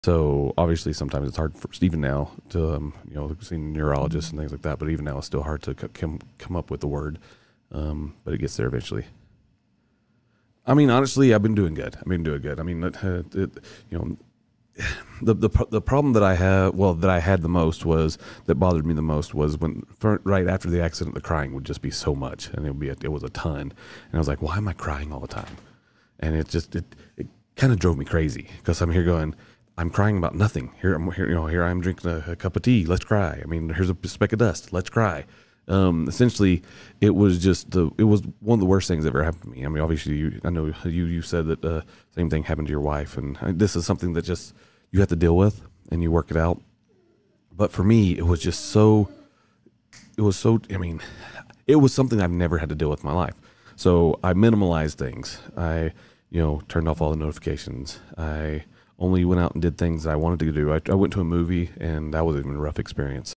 Overcoming Emotional Turmoil After Injury My Journey
The main character shares their emotional struggles and coping mechanisms after an injury.
So obviously sometimes it's hard, even now, seeing a neurologist and things like that, but even now it's still hard to come up with the word. But it gets there eventually. i mean honestly i've been doing good i've been doing good i mean you know the the problem that i have well that i had the most was that bothered me the most was when right after the accident the crying would just be so much and it was a ton and i was like why am i crying all the time And it just kind of drove me crazy because I'm here going, I'm crying about nothing. Here I am drinking a cup of tea. Let's cry. I mean, here's a speck of dust. Let's cry. Essentially, it was just one of the worst things that ever happened to me. I mean, obviously, I know you said that the same thing happened to your wife. And this is something that just you have to deal with and you work it out. But for me, it was just so I mean It was something I've never had to deal with in my life. So I minimalized things. I turned off all the notifications. I only went out and did things I wanted to do. I went to a movie, and that wasn't even a rough experience.